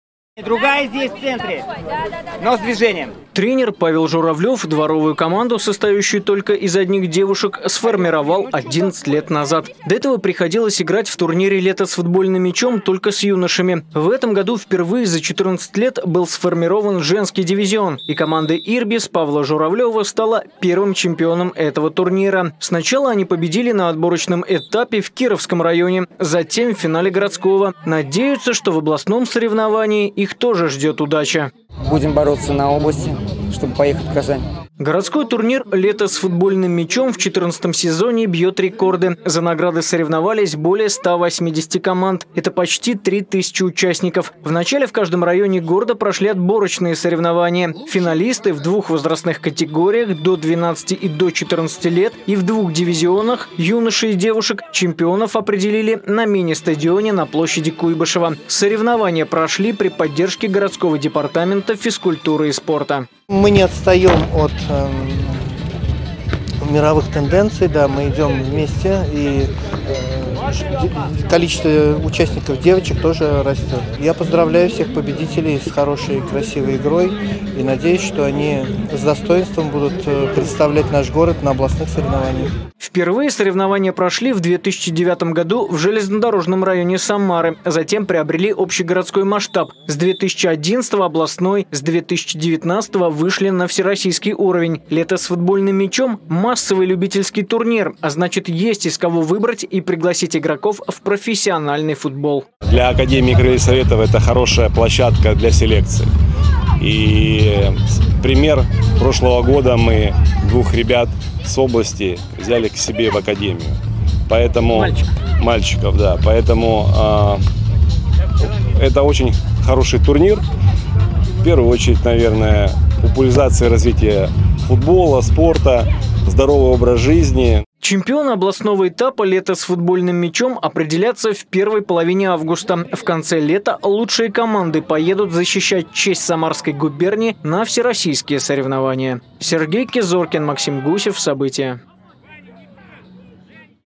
В Самаре наградили победителей турнира "Лето с футбольным мячом"
В Самаре определились победители и призеры любительского турнира "Лето с футбольным мячом". За награды городского этапа соревновались почти три тысячи юношей и девушек. В этом году впервые за 14 лет проводилось параллельно два турнира. Для юношей и девушек. До этого составы команд были смешанными. Подробнее расскажет Сергей Кизоркин.